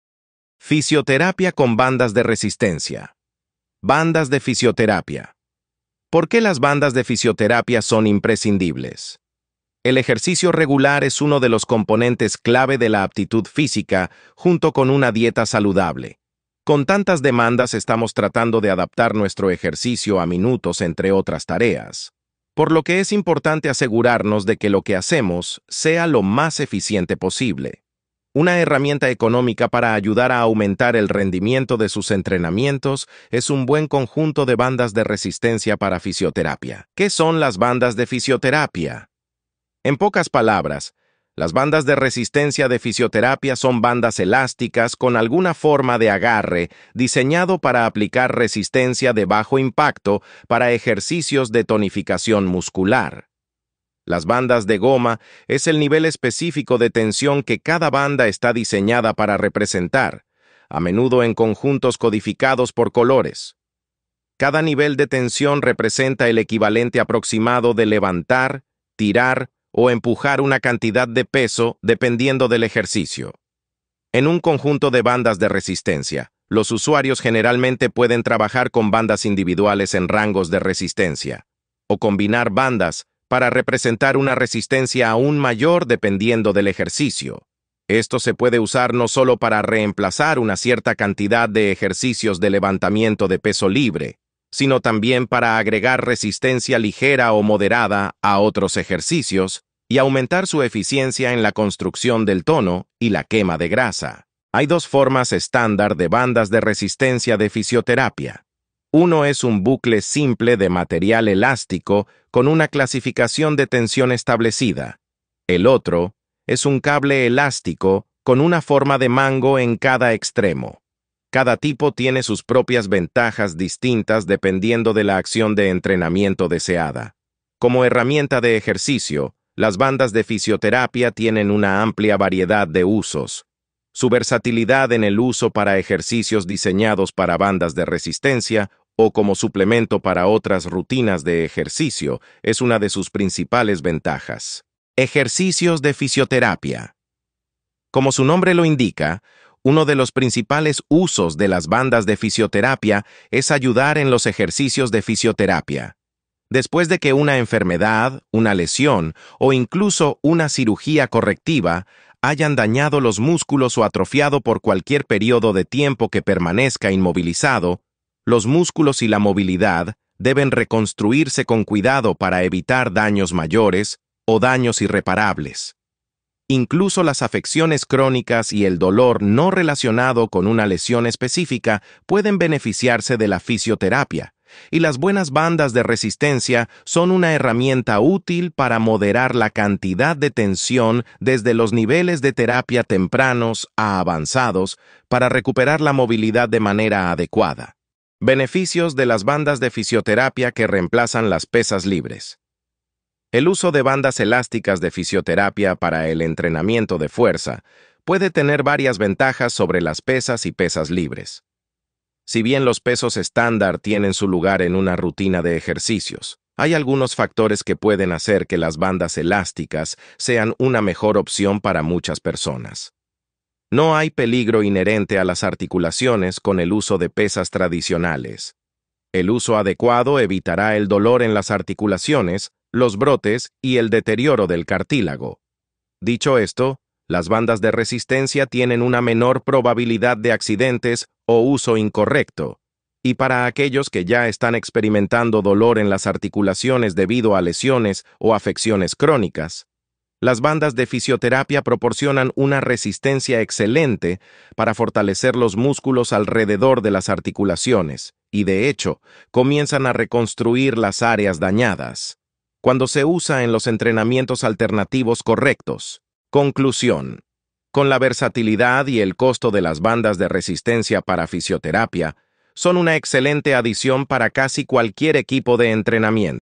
Fisioterapia Ejercicios de resistencia con banda elástica en paciente con secuela Neurológica
¿QUÉ SON LAS BANDAS DE FISIOTERAPIA?

En pocas palabras, las bandas de resistencia de fisioterapia son bandas elásticas con alguna forma de agarre diseñado para aplicar resistencia de bajo impacto para ejercicios de tonificación muscular.

Las bandas de goma es el nivel específico de tensión que cada banda está diseñada para representar, a menudo en conjuntos codificados por colores. Cada nivel de tensión representa el equivalente aproximado de levantar, tirar o empujar una cantidad de peso dependiendo del ejercicio.

En un conjunto de bandas de resistencia, los usuarios generalmente pueden trabajar con bandas individuales en rangos de resistencia, o combinar bandas para representar una resistencia aún mayor dependiendo del ejercicio.

Esto se puede usar no solo para reemplazar una cierta cantidad de ejercicios de levantamiento de peso libre, sino también para agregar resistencia ligera o moderada a otros ejercicios y aumentar su eficiencia en la construcción del tono y la quema de grasa.

Hay dos formas estándar de bandas de resistencia de fisioterapia. Uno es un bucle simple de material elástico con una clasificación de tensión establecida; el otro es un cable elástico con una forma de mango en cada extremo. Cada tipo tiene sus propias ventajas distintas dependiendo de la acción de entrenamiento deseada.

Como herramienta de ejercicio, las bandas de fisioterapia tienen una amplia variedad de usos. Su versatilidad en el uso para ejercicios diseñados para bandas de resistencia o como suplemento para otras rutinas de ejercicio es una de sus principales ventajas.